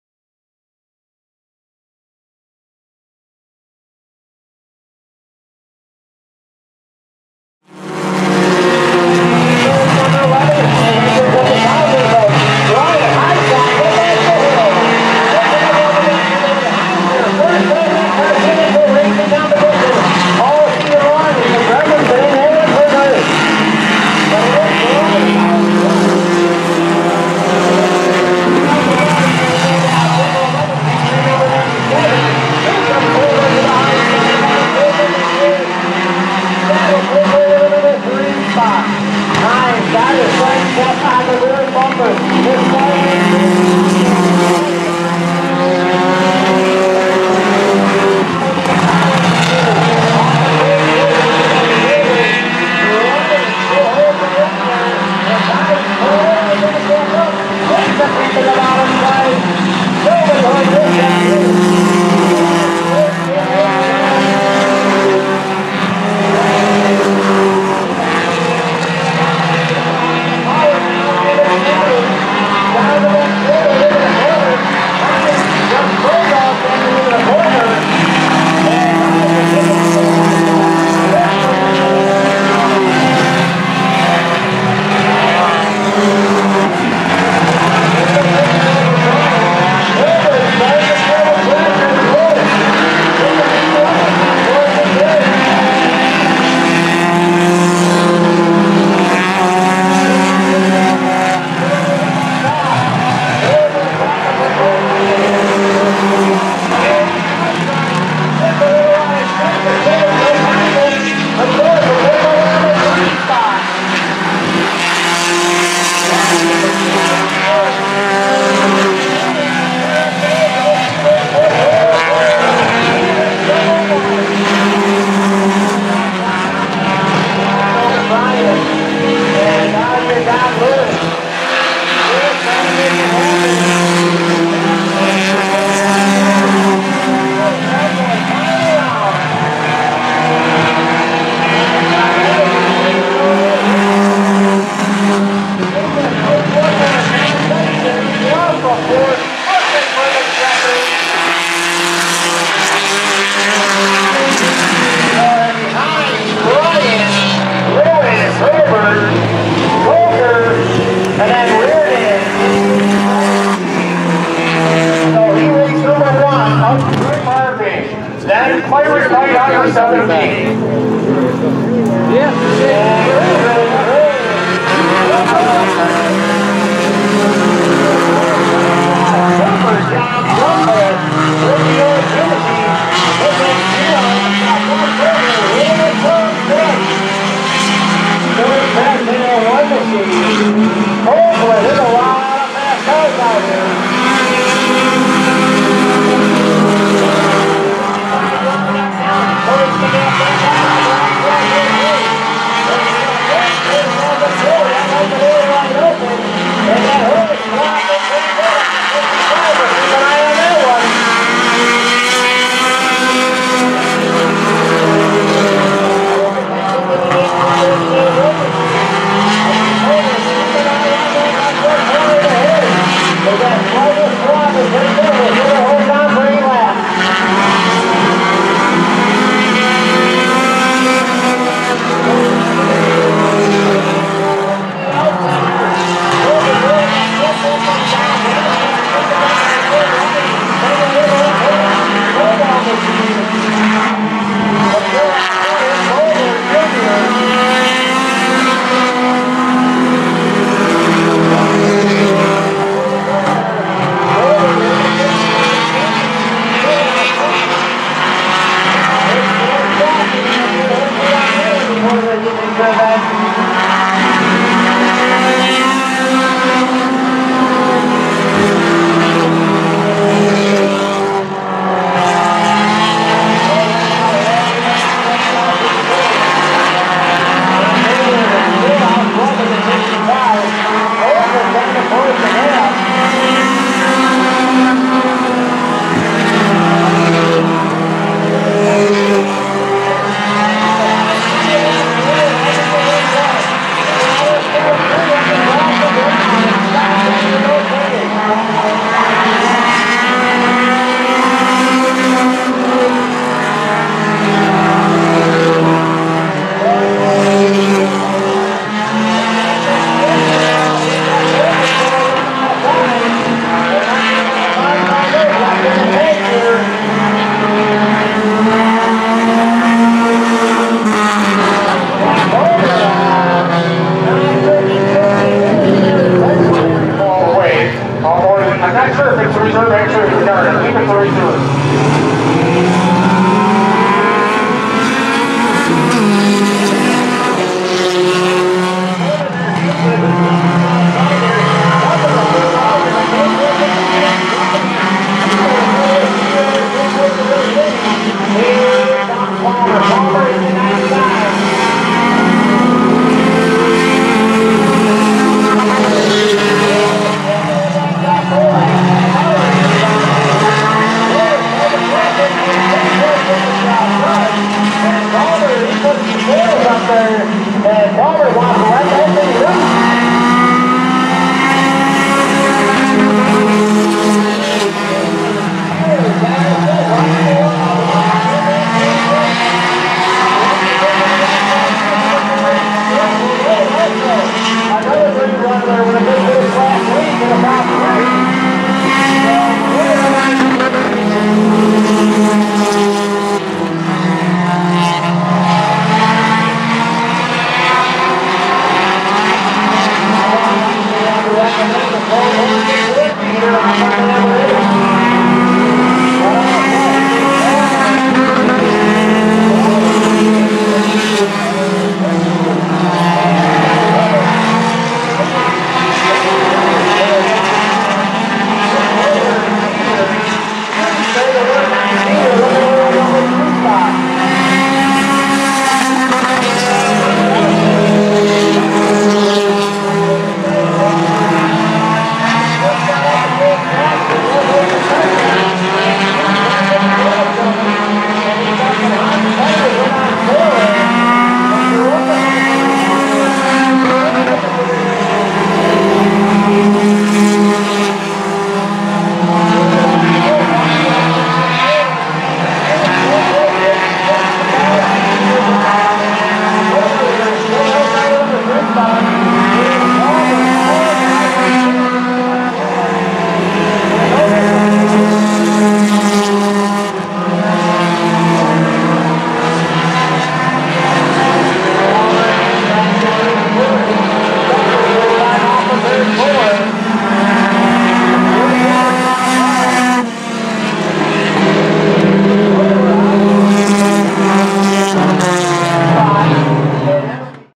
WCIS G DOG Pro 4 Cylinder Heats 6-26-21
The G DOG Pro 4 Cylinder Heats from Wyoming County Intl Speedway on 6-26-21!